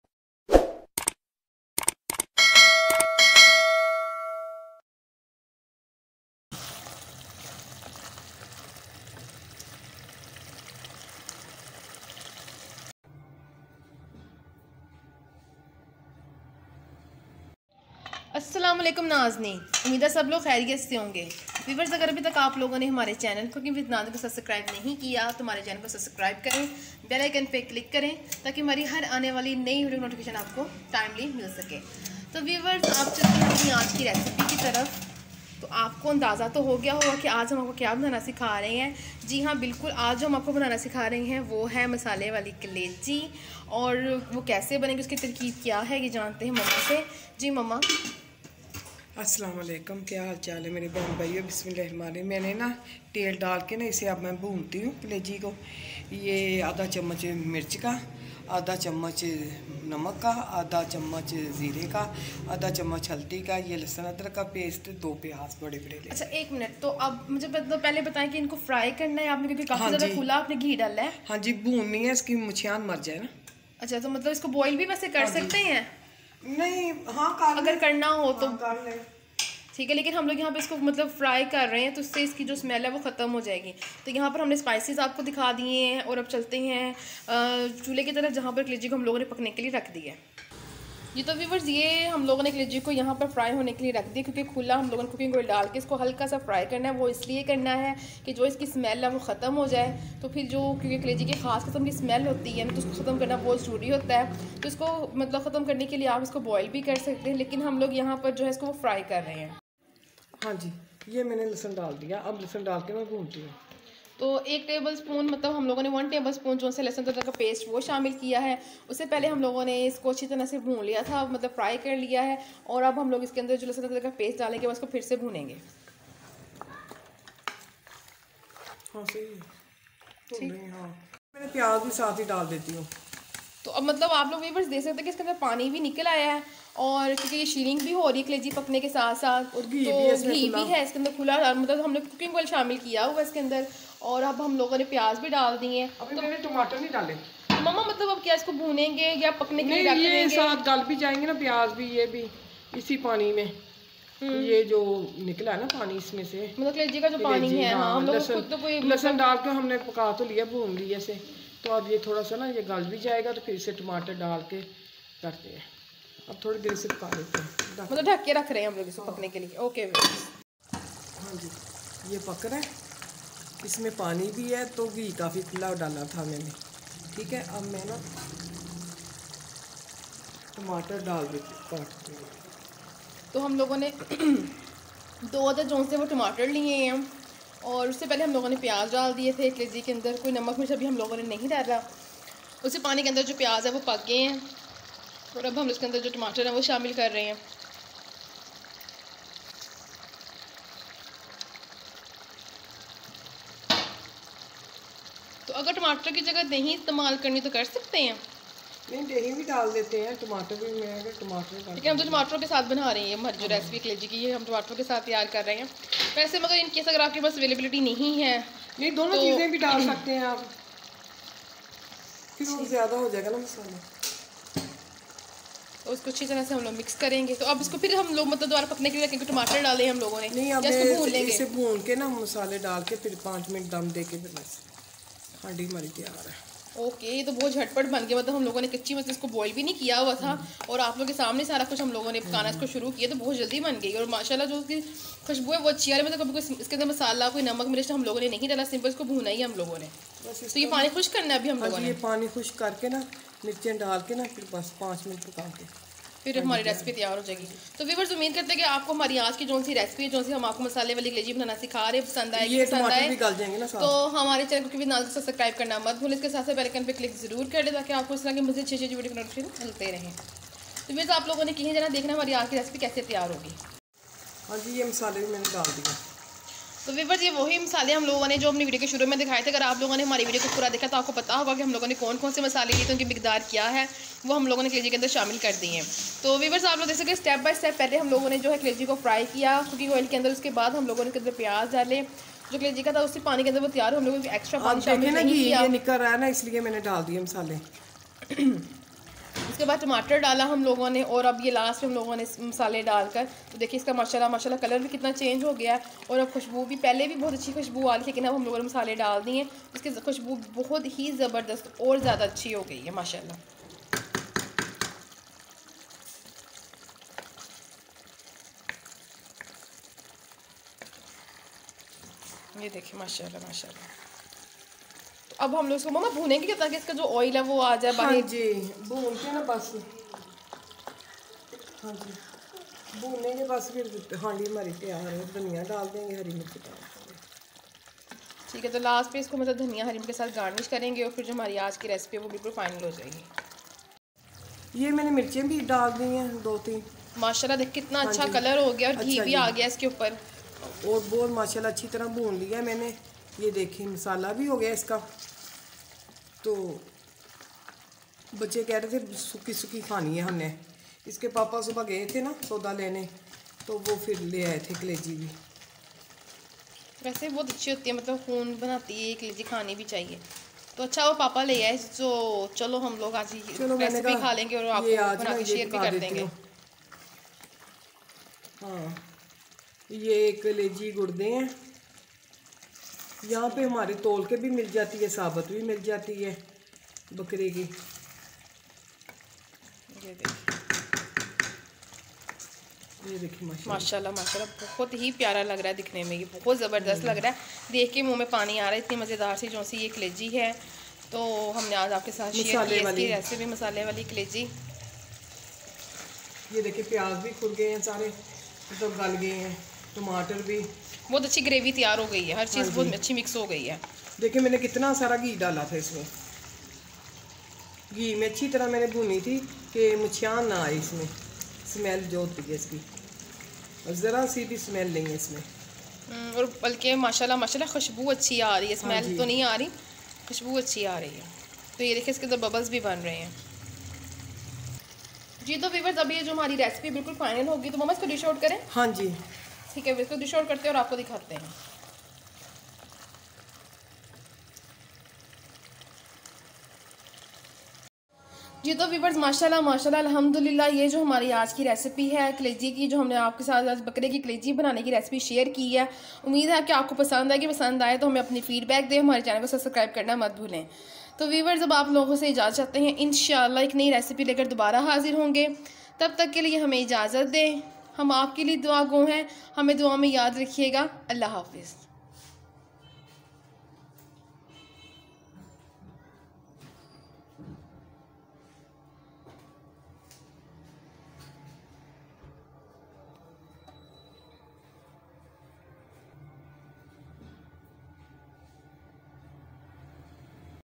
Easy & Tasty Fry Mutton Kaleji | Liver Masala |Kaleji Ka Salan |Dhaba Style Kaleji Recipe By Nazneen
Easy & Tasty Mutton Kaleji | Liver Masala | Kaleji Ka Salan | Dhaba Style Kaleji Recipe By Nazneen